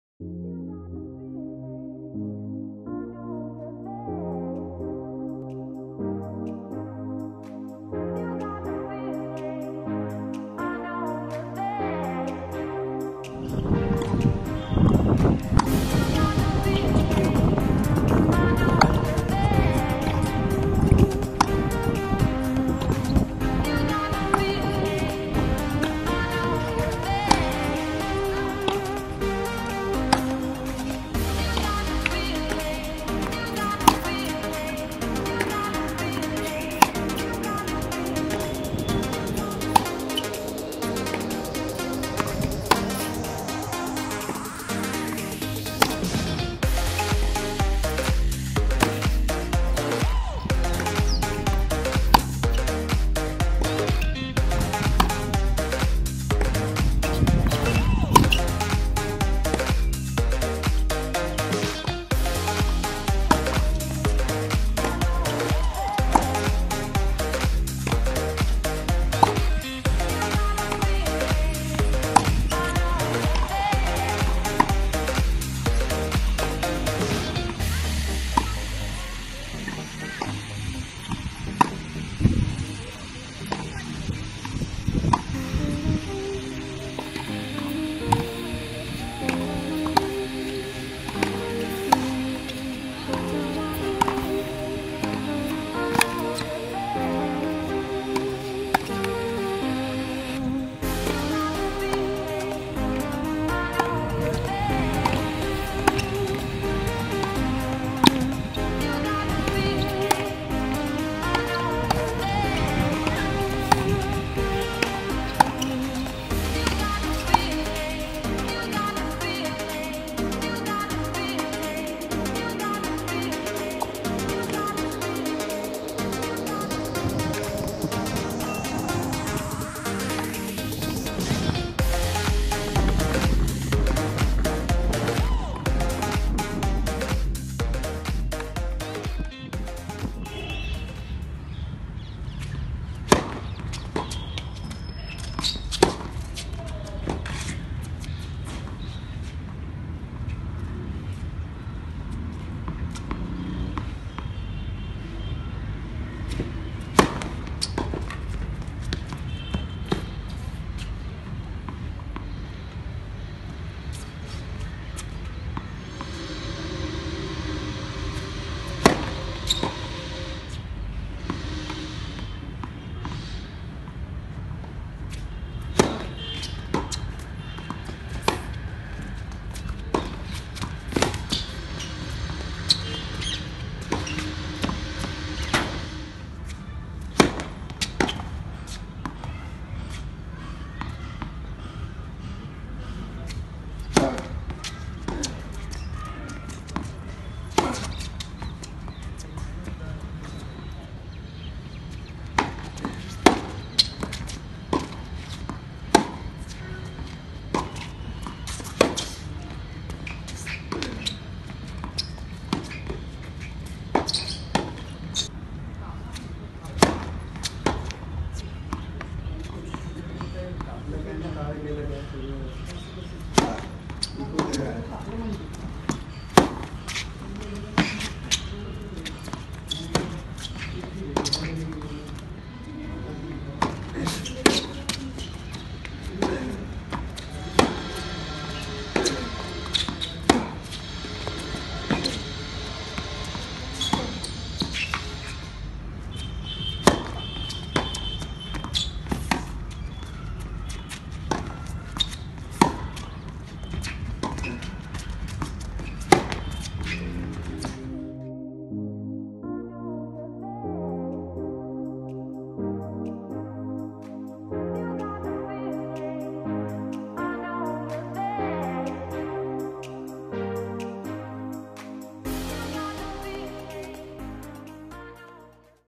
Piyush Salekar - USAthlete Services
College Tennis Recruit - Fall 2018
Name: Piyush Salekar
DoB: May 14th, 2000
Height/Weight: 6'3''/163 lbs
Citizenship(s): Indian
Ranking: #377 ITF